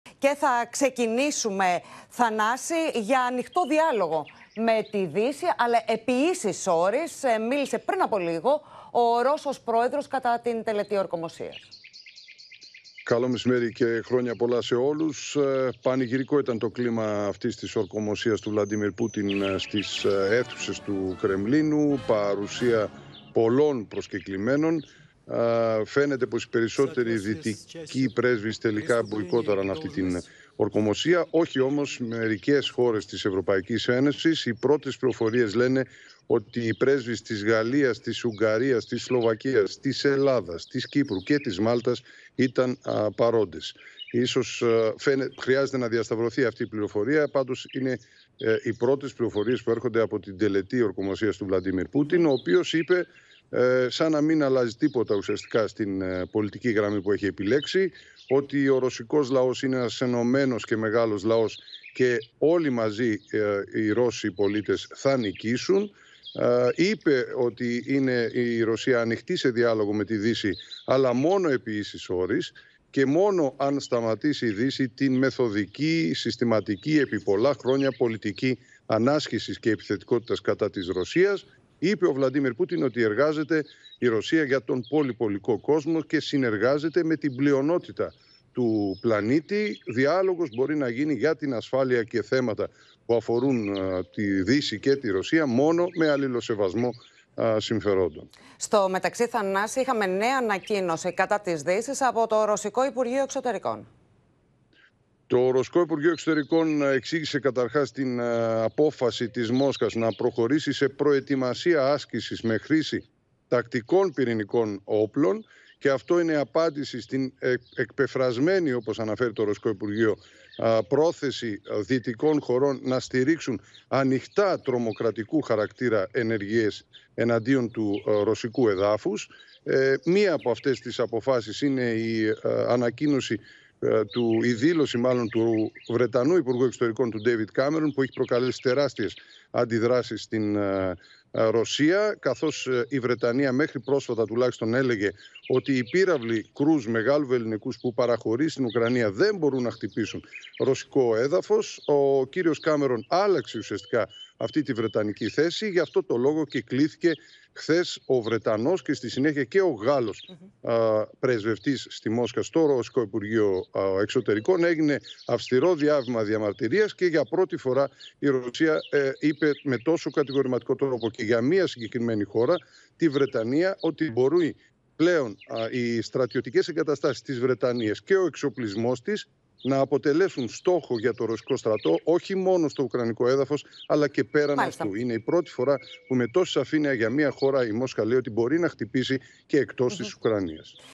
Πούτιν: Είμαστε ένας ενωμένος και μεγάλος λαός, μαζί θα νικήσουμε | OPEN TV
Για ανοιχτό διάλογο με τη Δύση αλλά επί ίσοις όροις έκανε λόγο πριν από λίγο στην ομιλία του ο Ρώσος πρόεδρος κατά την τελετή ορκωμοσίας.